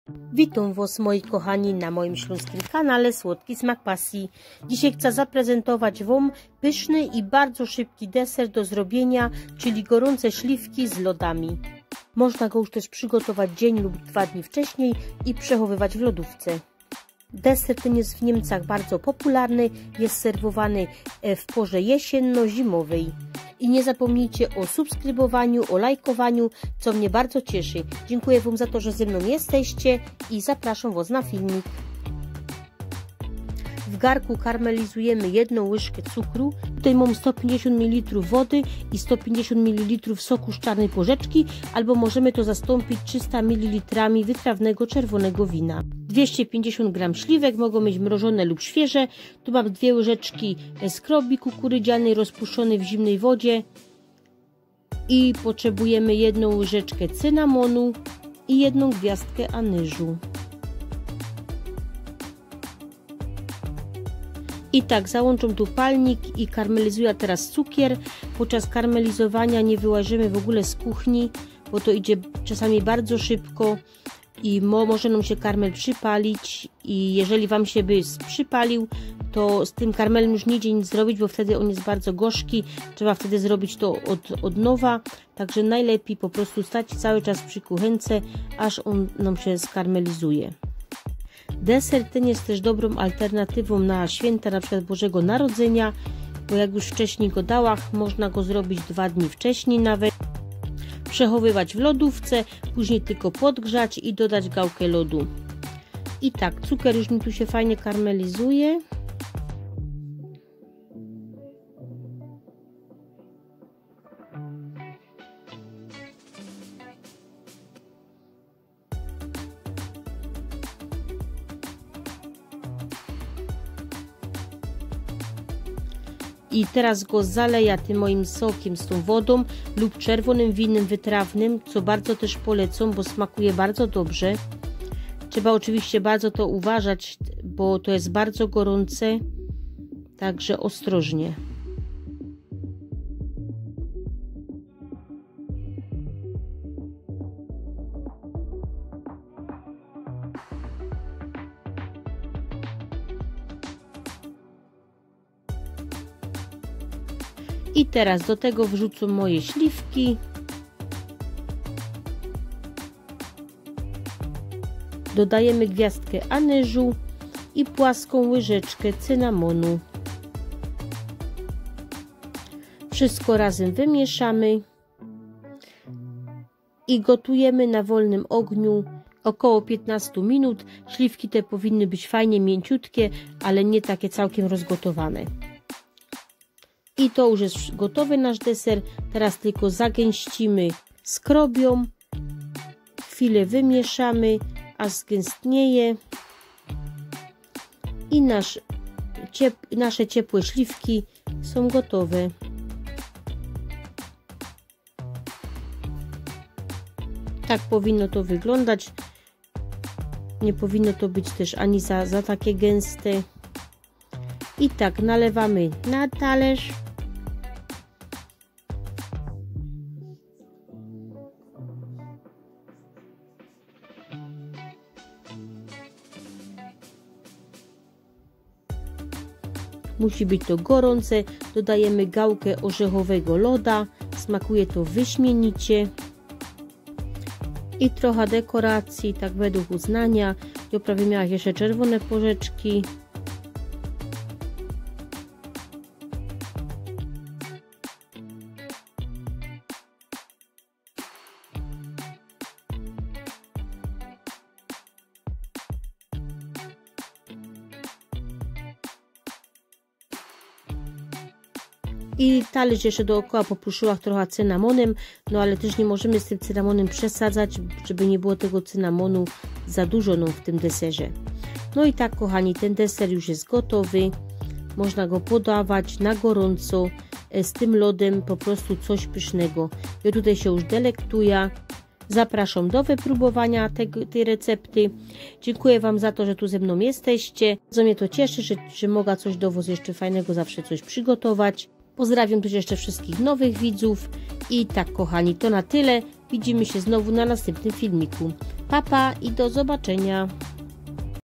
Deser na Wigilię Święta Śliwki na gorąco z lodami
Składniki na 2 porcje:

250g śliwek świeżych lub mrożonych
1 łyżka cukru
150ml wody
150ml soku z czarnej porzeczki
lub 300ml czerwonego wytrawnego wina
1 gwiazdka anyżu
1 płaska łyżeczka cynamonu
2 płaskie łyżeczki skrobi kukurydzianej
1 gałka loda orzechowego